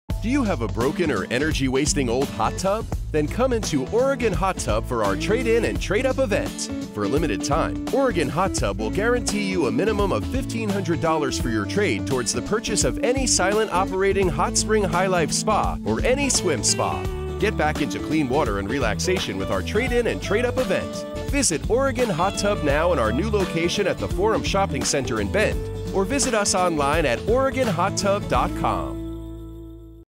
Oregon Hot Tub Trade In & Trade Up Event
For a limited time, get a minimum of $1,500 on your trade in when you purchase select hot tubs, or any swim spa. Trade in your old or broken hot tub or swim spa and we'll give you AT LEAST $1,500!

This event is being celebrated at our new Bend location, at The Forum Shopping Center across from Barnes & Noble.